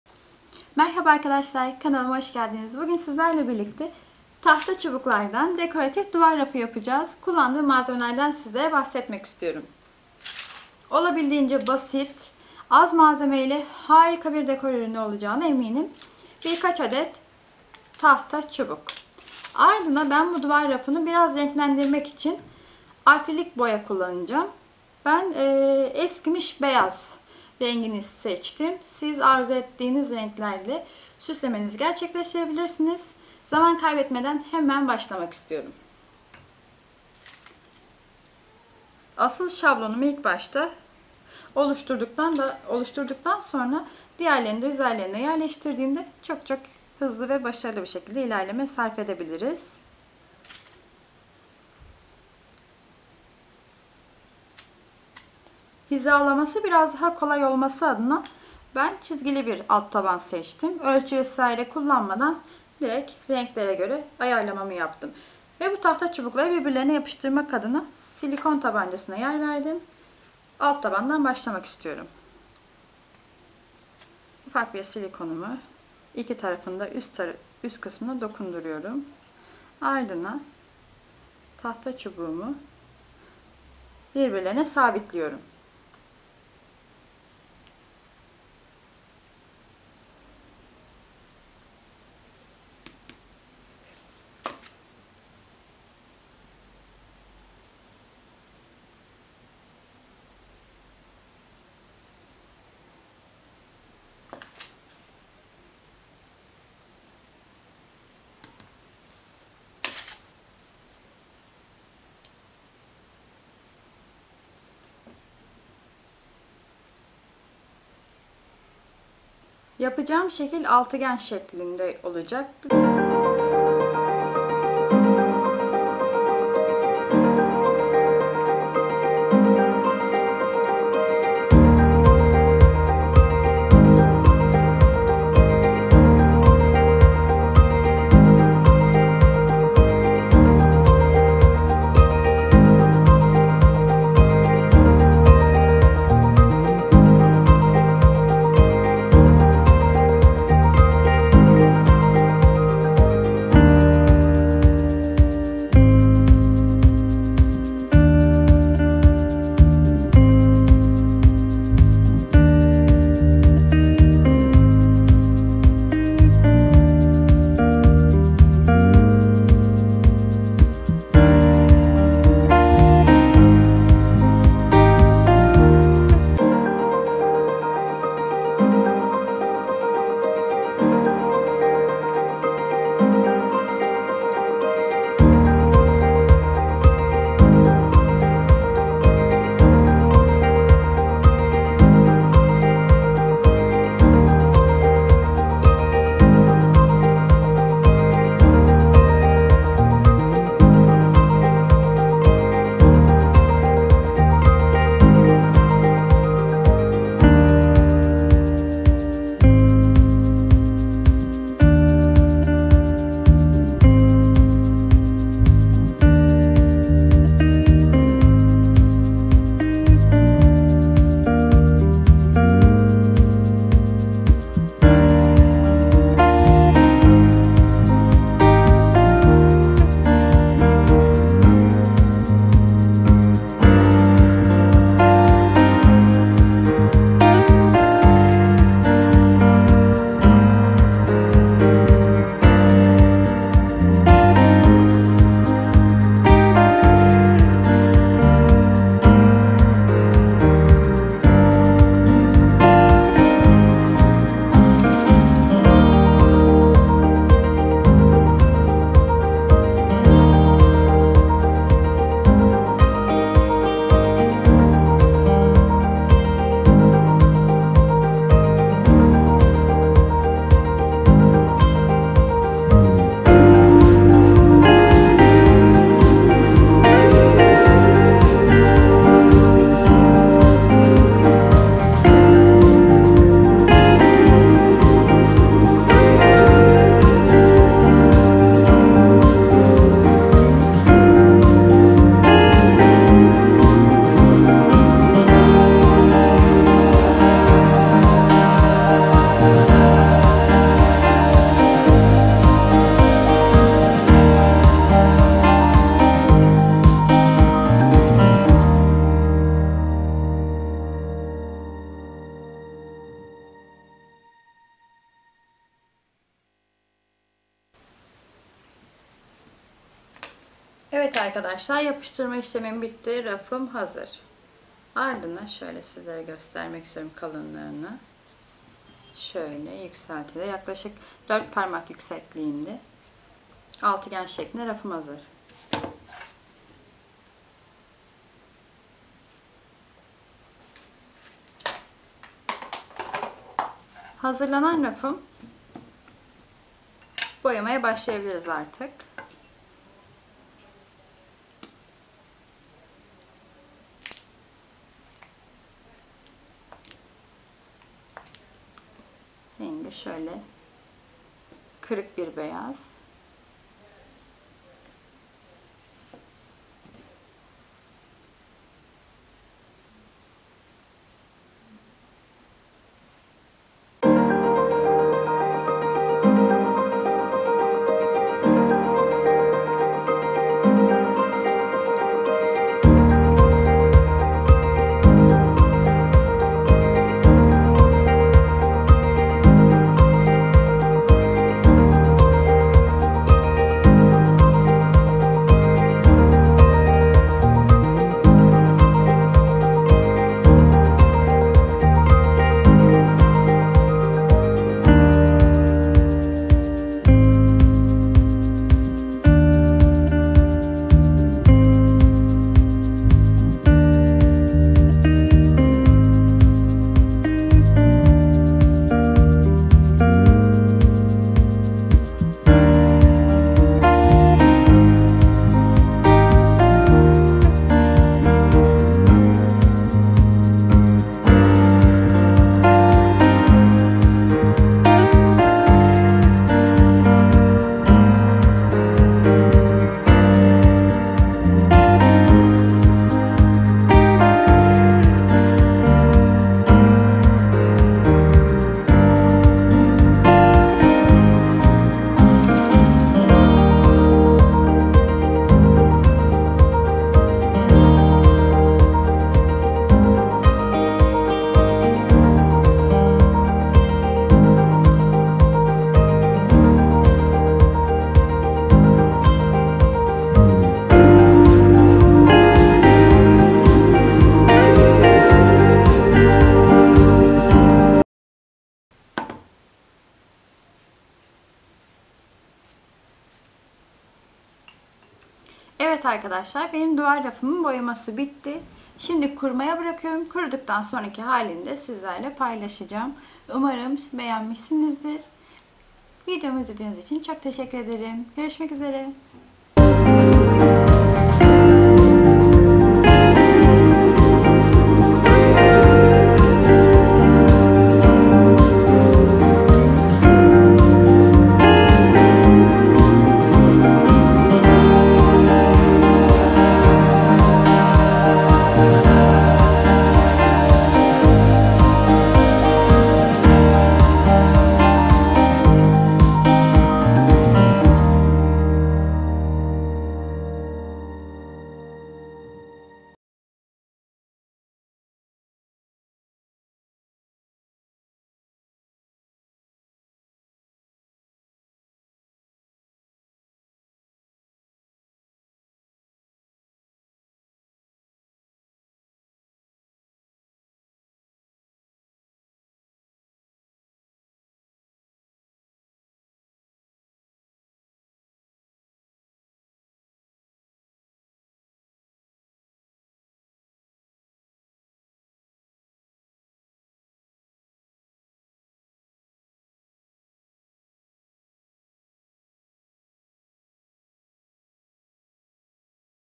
Tahta Çubuklardan Dekoratif Raf
Merhaba arkadaşlar sizler için tahta çubuklardan dekoratif ve yapımı çok basit olan arka plan, duvar rafı hazırladım.
Yapım aşamasında oldukça keyif aldım, umarım sizlerde izlerken ve uygulamada memnun kalırsınız.
İyi seyirler.